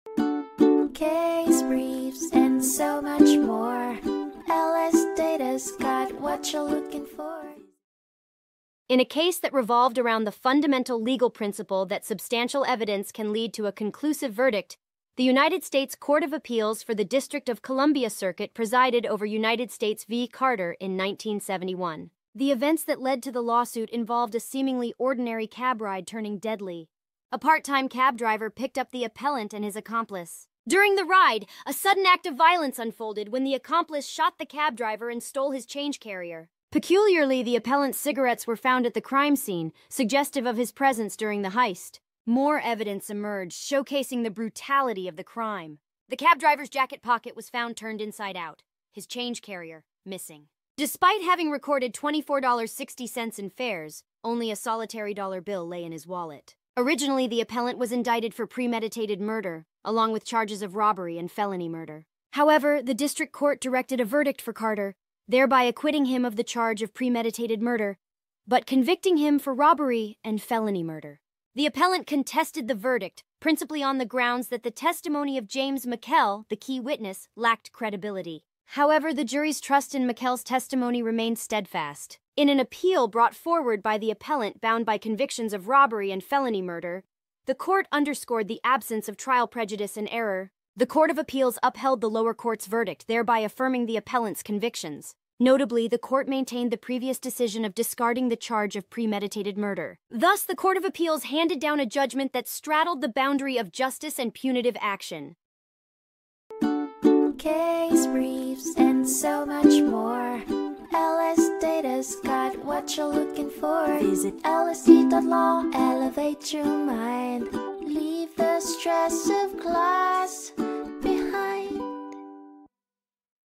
United States v. Carter (1971) Overview | LSData Case Brief Video Summary
The appellant was charged with robbery, premeditated murder, and felony murder. He was acquitted of premeditated murder but convicted of robbery and felony murder. The appellant and his accomplice shot a part-time cab driver and stole his change carrier. The victim's jacket pocket was turned inside out, and his change carrier was missing. The appellant's Pall Malls were found on the dashboard of the cab. The victim's record of fares for the day totaled $24.60, but only a one-dollar bill was found in his wallet. The appellant's main argument on appeal is that James Makel's testimony is not credible, but the jury believed it. Makel's testimony implicated the appellant in the robbery.

United States v. Carter (1971)
United States Court of Appeals for the District of Columbia Circuit
445 F.2d 669